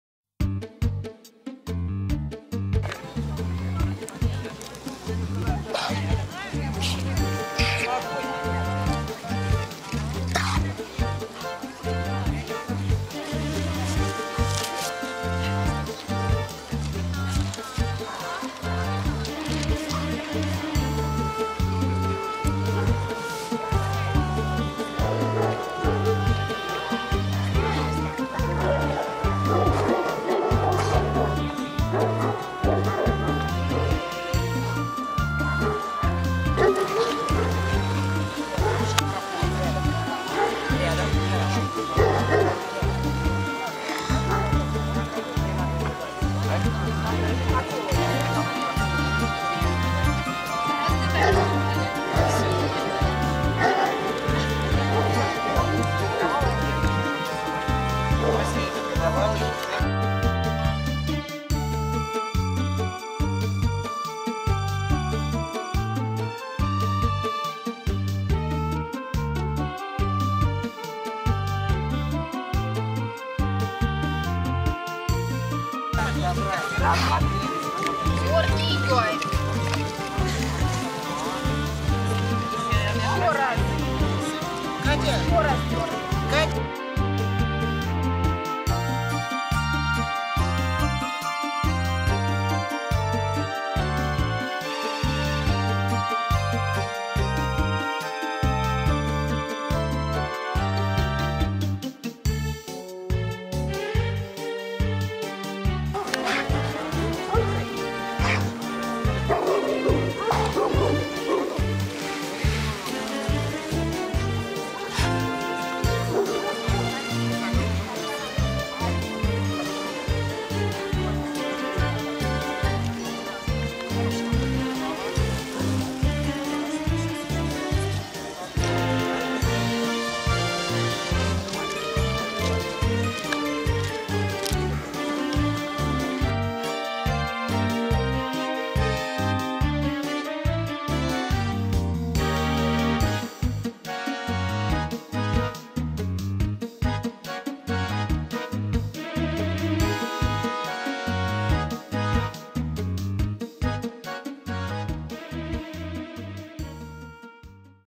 Moscow Watchdog Чемпионат 2012 ч XVIII c. Открытый кл.
Московская сторожевая

Чемпионат породы МОСКОВСКАЯ СТОРОЖЕВАЯ-2012
часть ХVIII

Монопородная выставка
ранг Чемпион Клуба. Эксперт РКФ-FCI Никитин А.В. (Россия, Воронеж)
Суки Класс ОТКРЫТЫЙ

30 апреля 2012
КПЦ "Атаман"